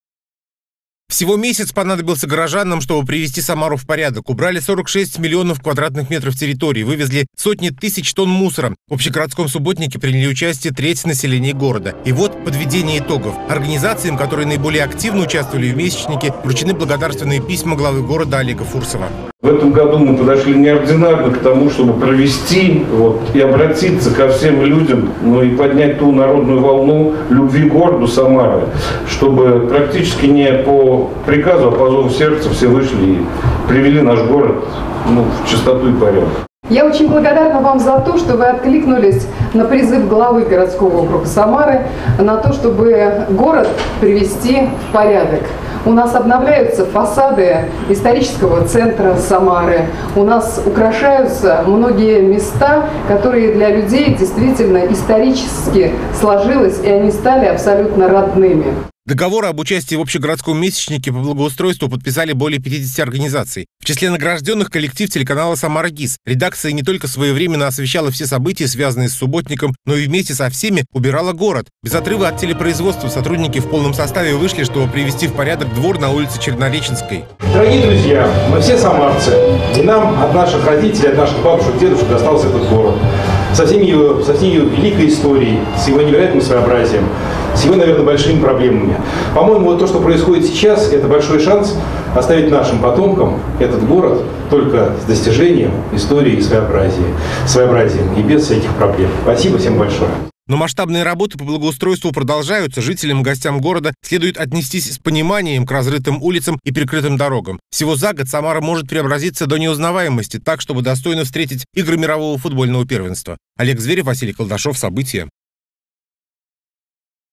Поработал отдохни. Передовики городских субботников получили благодарность от главы Самары
Месячник по благоустройству стал самым массовым за поселение годы. Почти полмиллиона человек вышли на общегородской субботник. Причём, многие организации работали в полном составе, а наиболее активных наградили в торжественной обстановке.